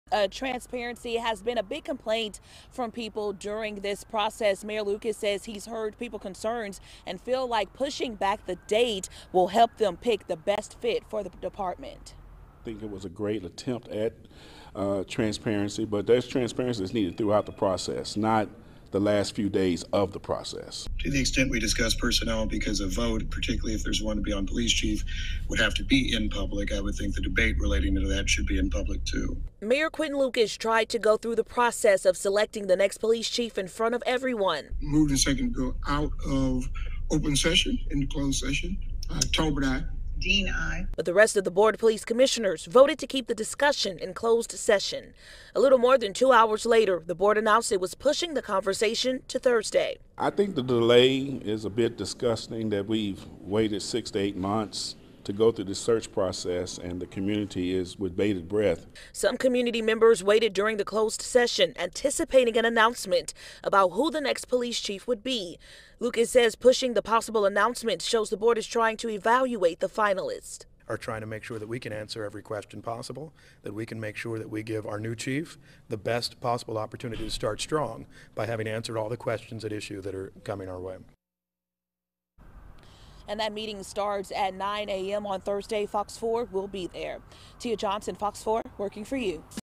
Some frustrated as police board delays announcement for new KCPD chief
Mayor Quinton Lucas tried to go through the process of selecting the next police chief in front of everyone. But the rest of the board of police commissioners voted to keep the discussion in closed session.